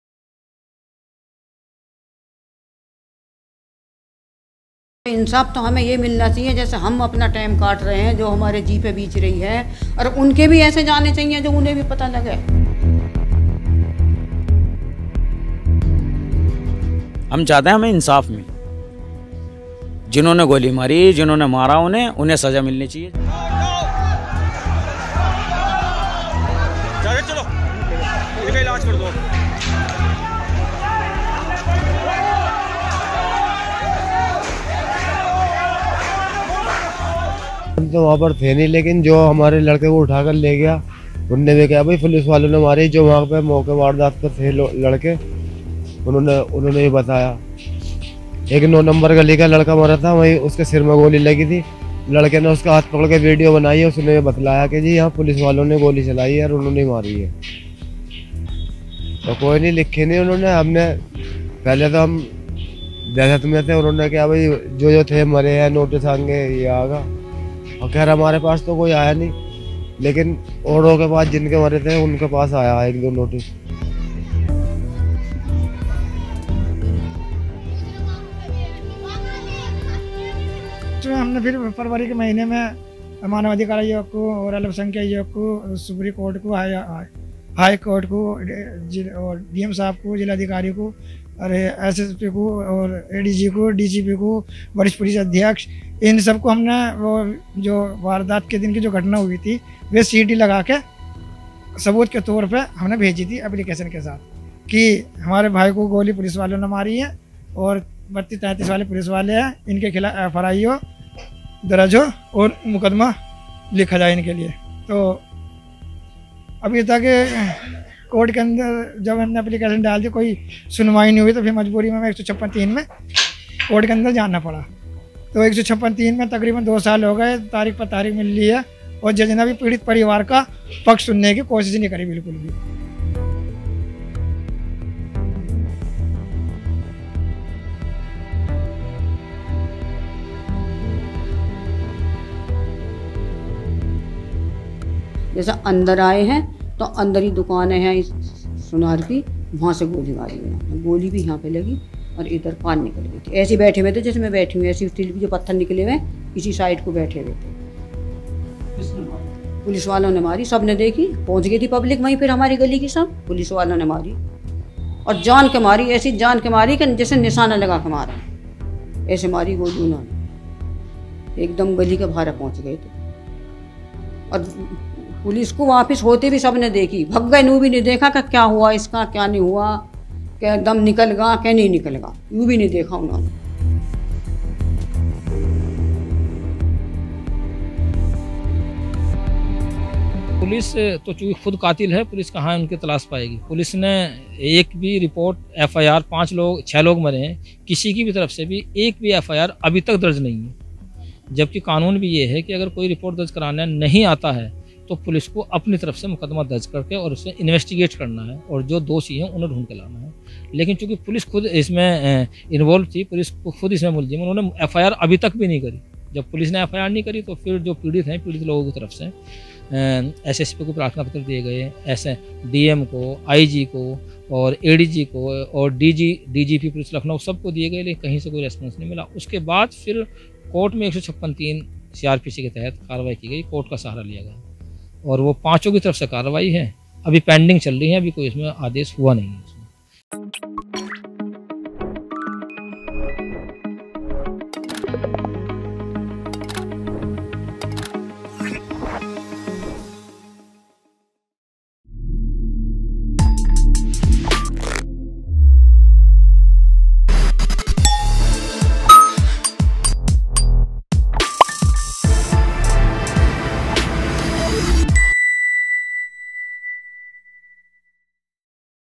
Ground Report | Family Members Allege Role of Police in Muslim Deaths during Anti-CAA Protests
The Anti-CAA protests which rocked the nation in December 2019, turned violent in many cities, including Uttar Pradesh’s Meerut, where six Muslims died during the clashes with the police officers. The family members accuse the police officers of being murderers and complain of no FIR being filed against them for two years now.